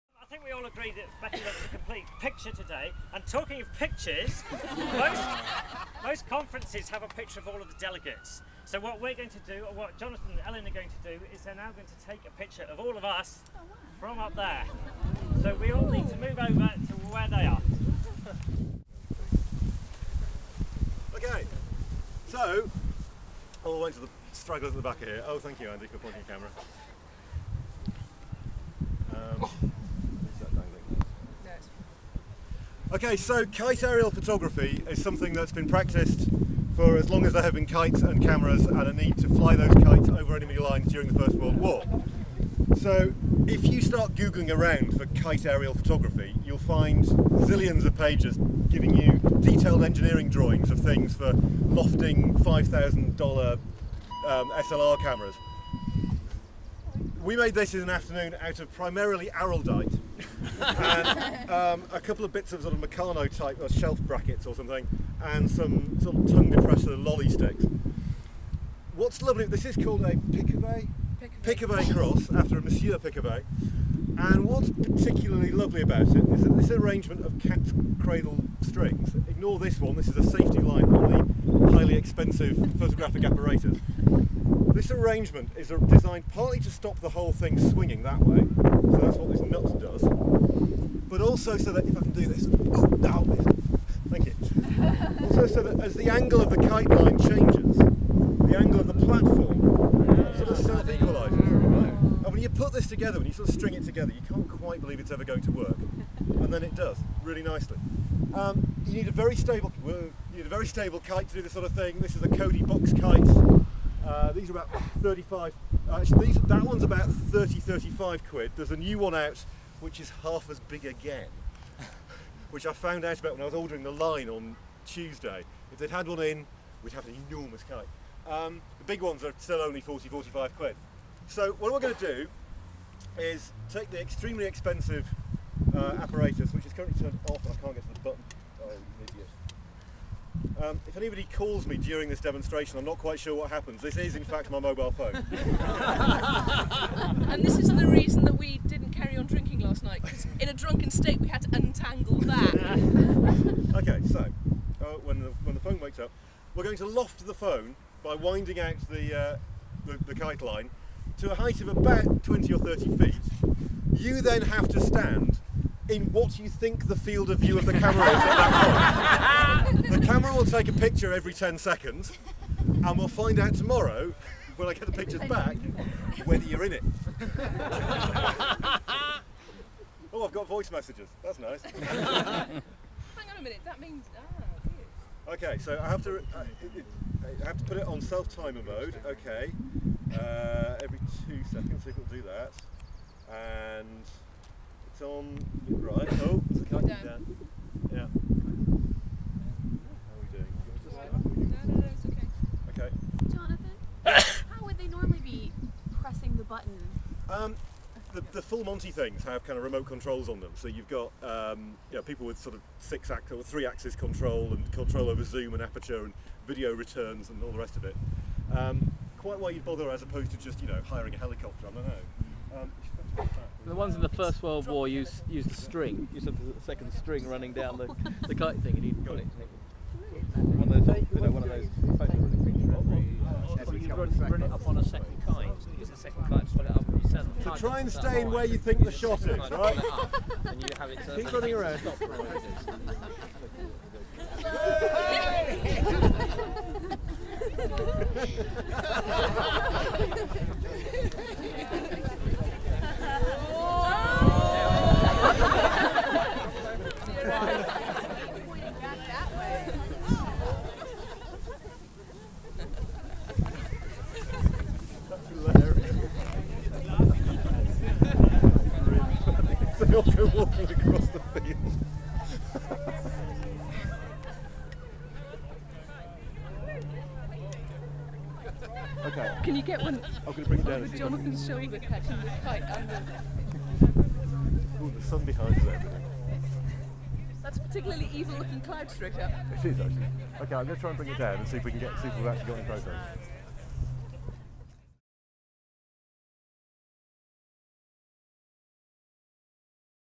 Kite photography
Demonstrating the rig used to suspend the camera from a kite line for the group photograph attempt at the BIG Event 2008. This first attempt wasn't entirely successful the other kite video in this series is the second, video-based, attempt.